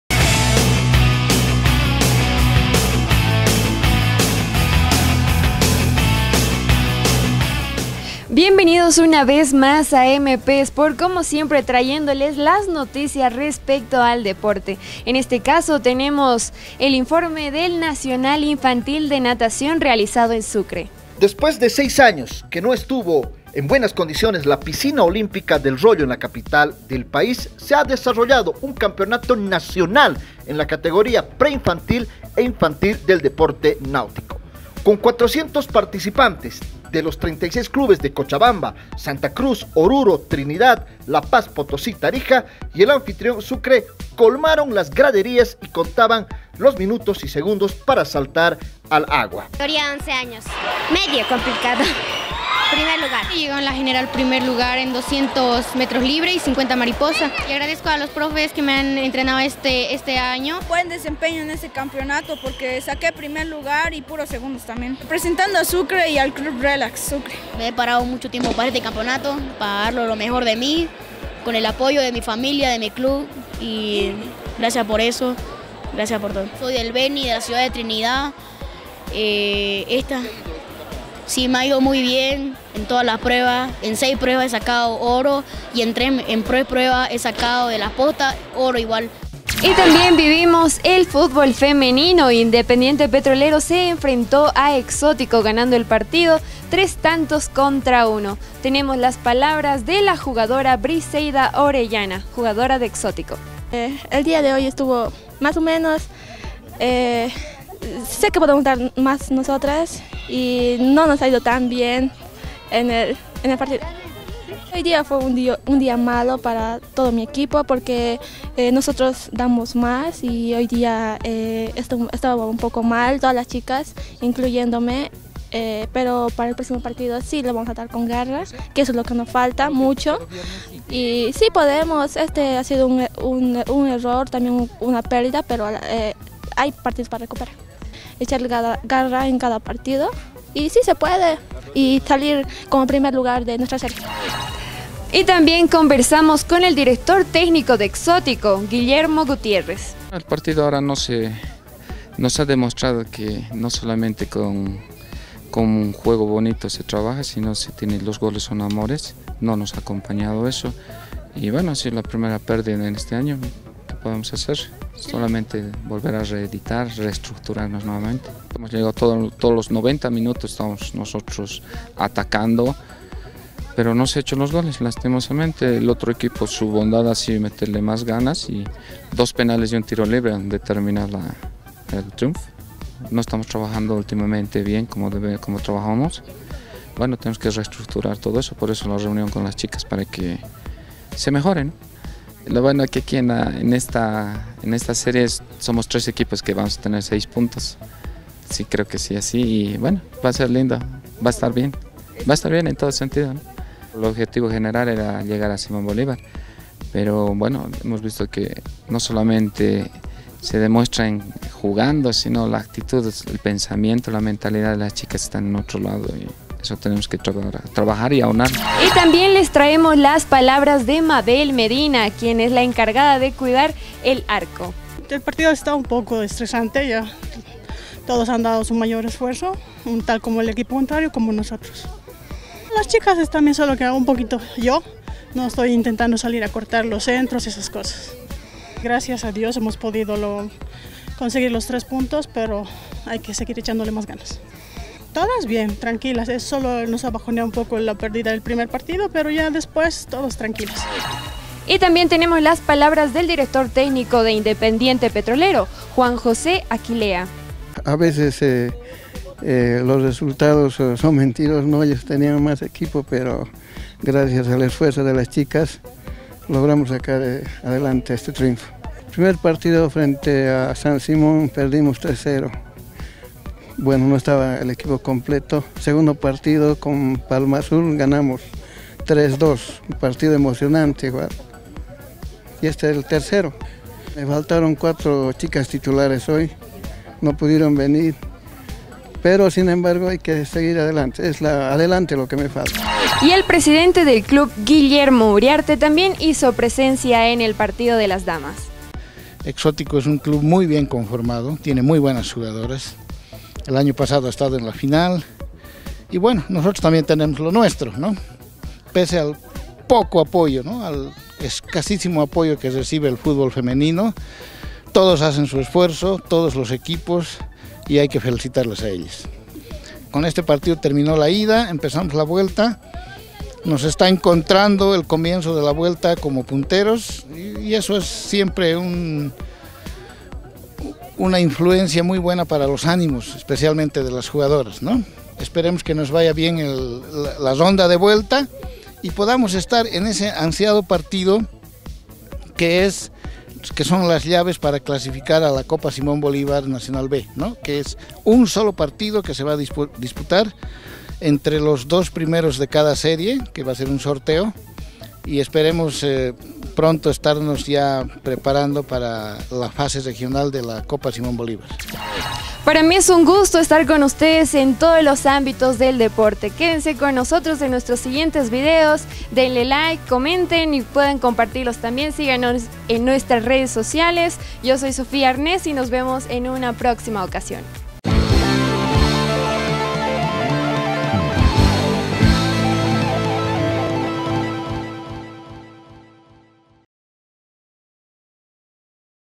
bienvenidos a MPSPORTS
nacional de natación infantil en Sucre, Chuquisaca
termino la fase de ida en el campeonato femenino Petrolero gano a Exotico por tres tantos a uno